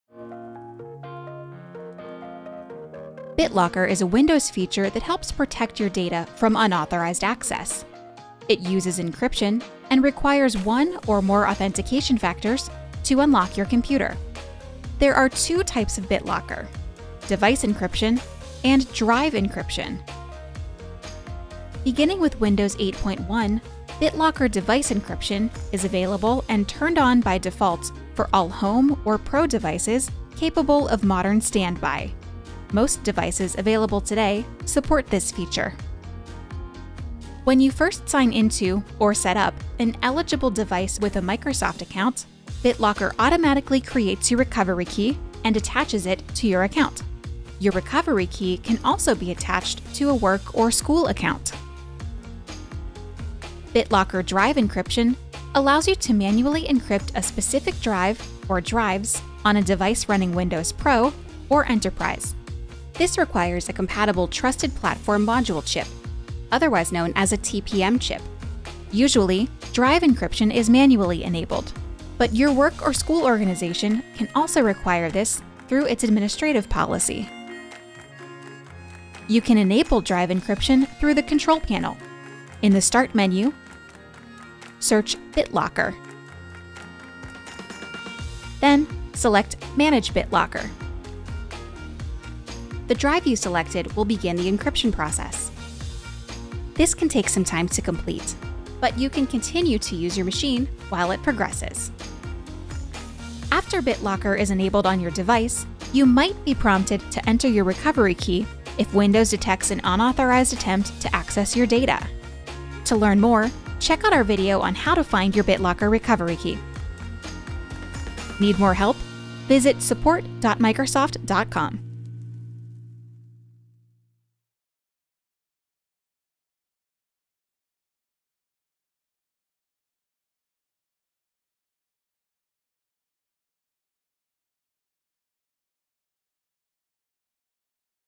What is BitLocker and BitLocker recovery?
BitLocker is the Windows encryption technology that protects your data from unauthorized access by encrypting your drive and requiring one or more factors of authentication before it will unlock it.

Windows will require a BitLocker recovery key when it detects a possible unauthorized attempt to access the data. This extra step is a security precaution intended to keep your data safe and secure. This can also happen if you make changes in hardware, firmware, or software which BitLocker cannot distinguish from a possible attack. In these cases, BitLocker may require the extra security of the recovery key even if the user is an authorized owner of the device. This is to be certain that the person trying to unlock the data really is authorized.

In this video, you will learn about the different types of BitLocker encryption, why BitLocker is used, and how you can activate BitLocker on your device.

0:00 Introduction
0:15 Types of BitLocker
0:20 BitLocker Device Encryption
0:36 What is a BitLocker Recovery Key?
0:52 BitLocker Drive Encryption
1:18 How to enable BitLocker Drive Encryption
1:42 BitLocker recovery key